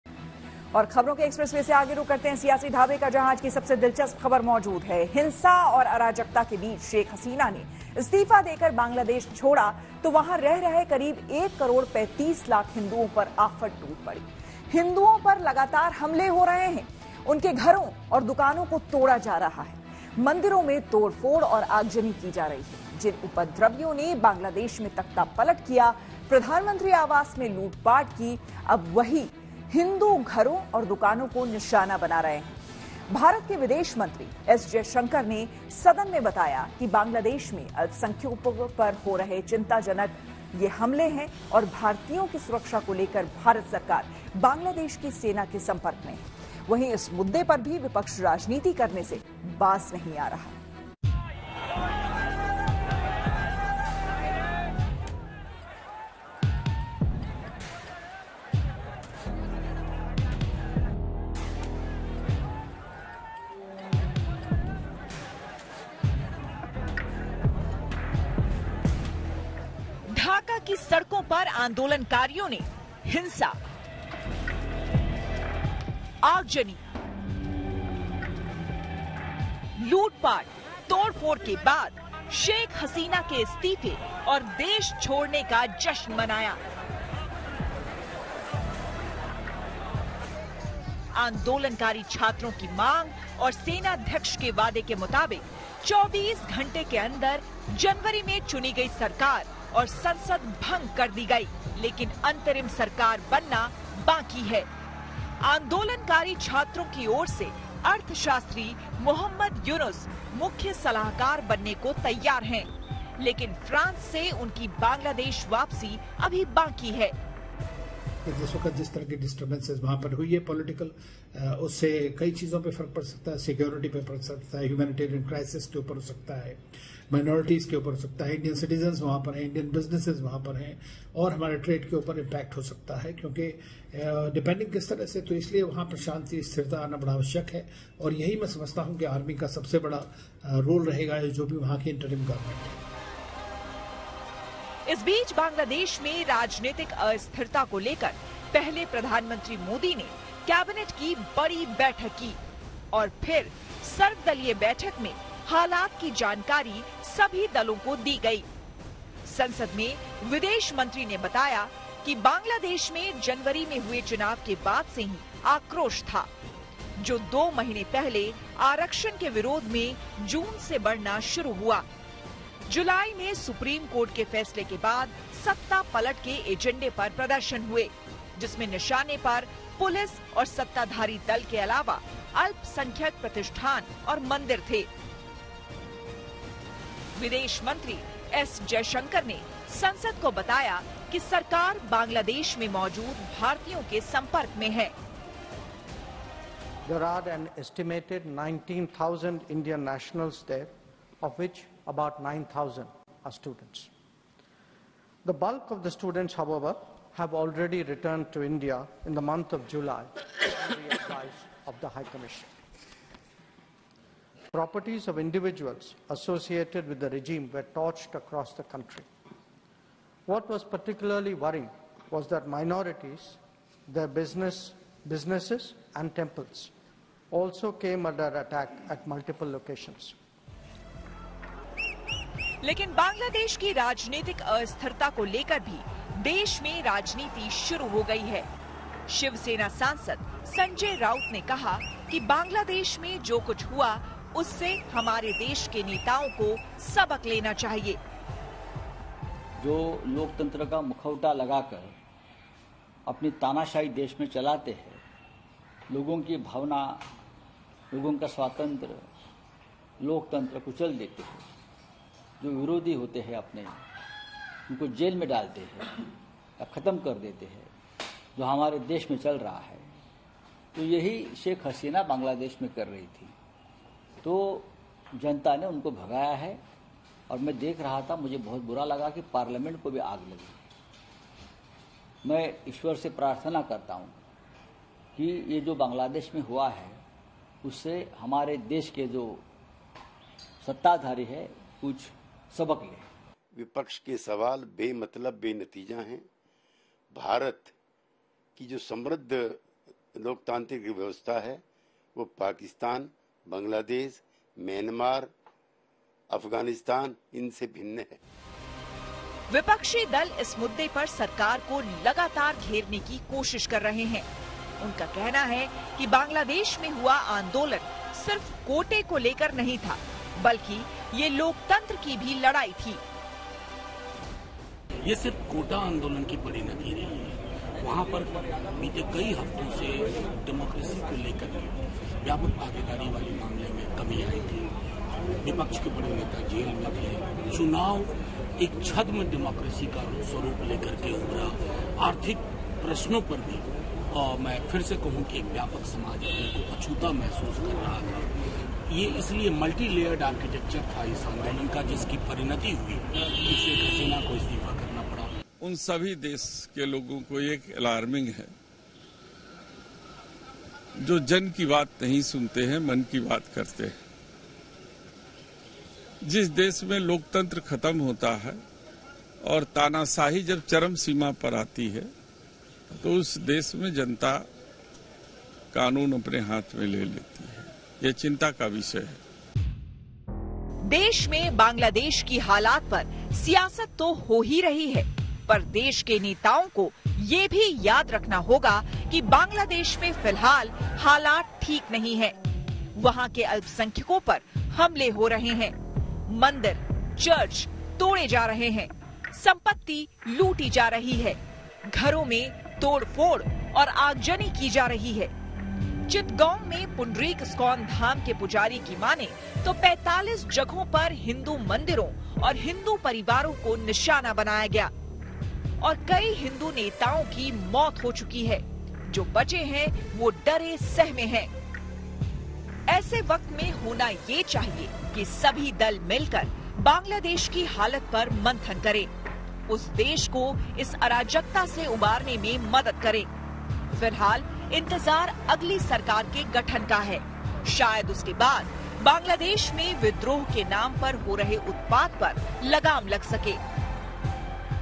Bangladesh की स्थिति पर India की है पैनी नजर, जानिए किस राजनेता ने क्या कहा
बांग्लादेश की राजनीतिक अस्थिरता को लेकर भारत में राजनीति शुरू हो गई है. शिवसेना सांसद संजय राउत ने कहा कि बांग्लादेश में जो कुछ हुआ, उससे हमारे देश के नेताओं को सबक लेना चाहिए. विपक्षी दल इस मुद्दे पर सरकार को लगातार घेरने की कोशिश कर रहे हैं. उनका कहना है कि बांग्लादेश में हुआ आंदोलन सिर्फ कोटे को लेकर नहीं था, बल्कि ये लोकतंत्र की भी लड़ाई थी.